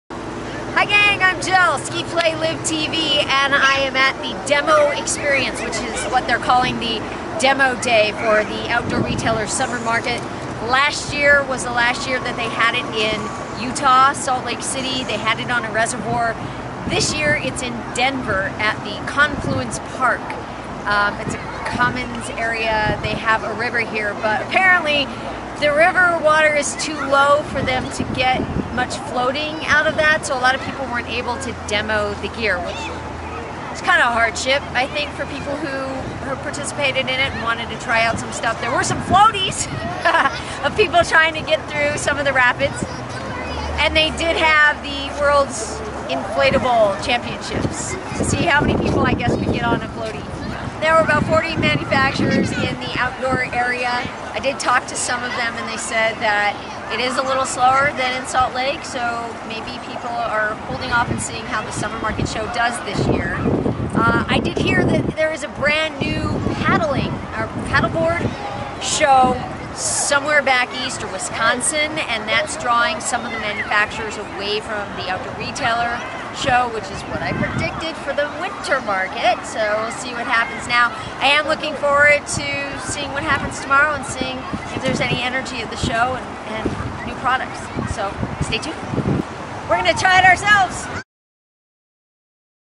Thoughts on Denver's First Outdoor Retailer Demo Day
I headed to Denver for their first Outdoor Retailer Summer market. It was a nice day by the river but it may take some time for this demo day to grow.